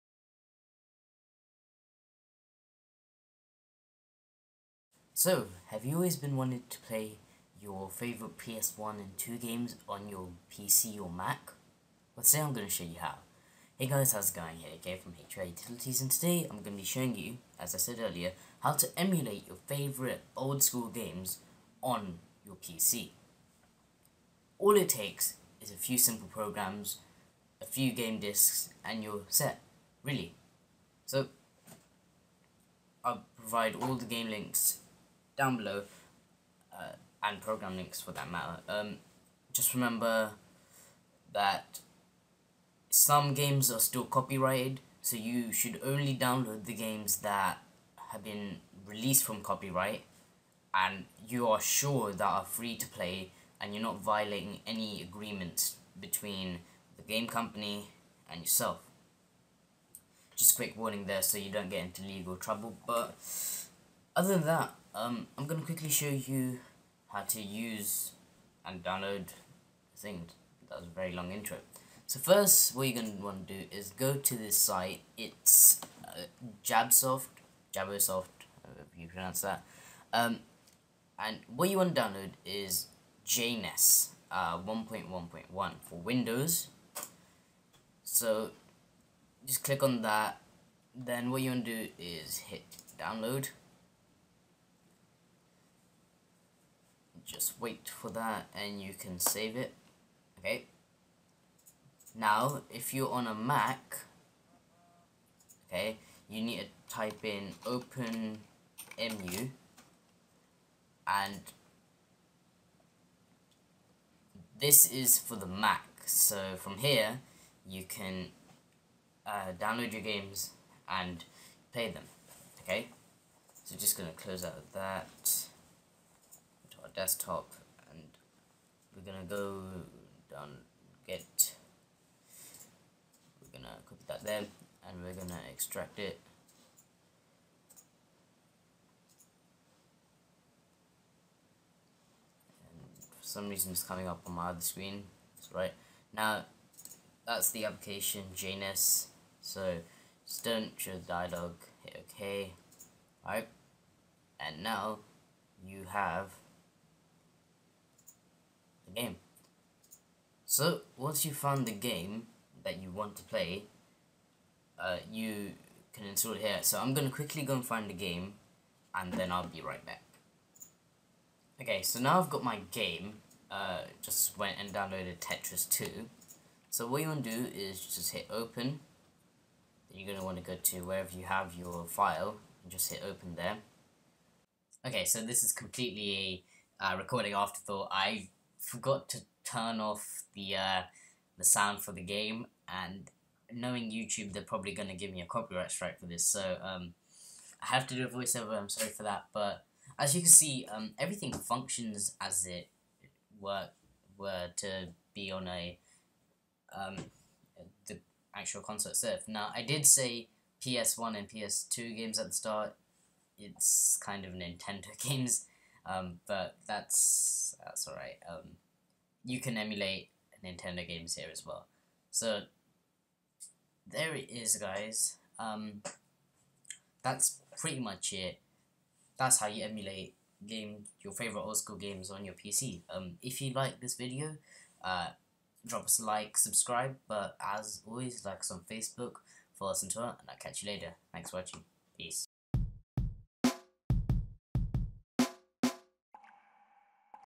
How To: Emulate Your Favourite Classic Games On Windows
So You've Got A PC And Want To Relive Those Childhood Memories, Right? Well Today I'll Show You How To Emulate Those Old-School Games On Your Mac/PC. It's Very Simple And Doesn't Take Up Much Space!